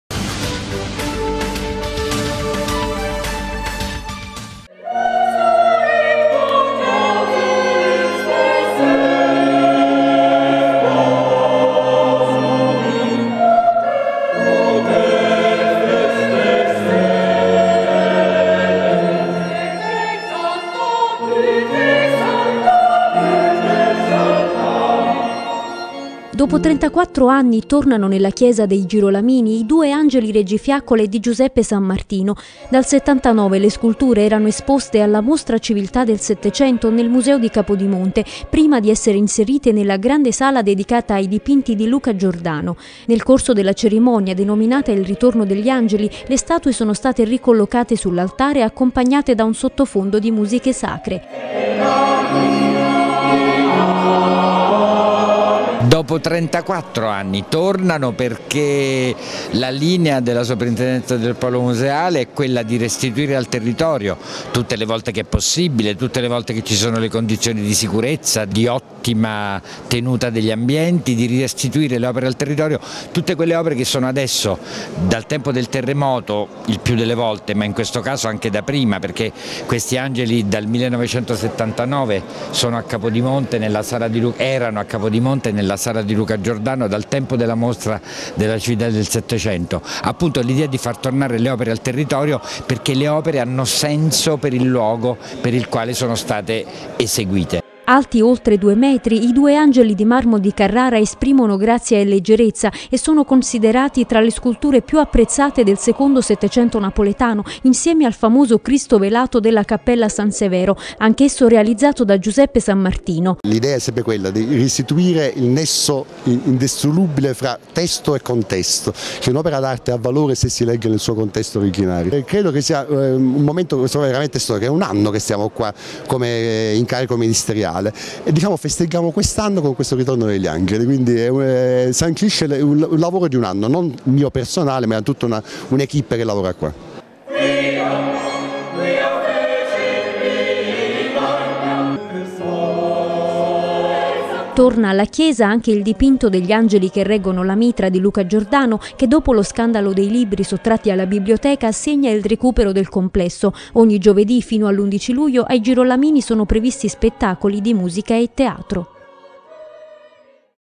Tornano ai Girolamini gli Angeli del Sanmartino
Dal 1979 erano in esposizione al Museo di Capodimonte. Oggi i due angeli di marmo scolpiti da Giuseppe Sanmartino tornano alla loro sede originaria nella Chiesa dei Girolamini.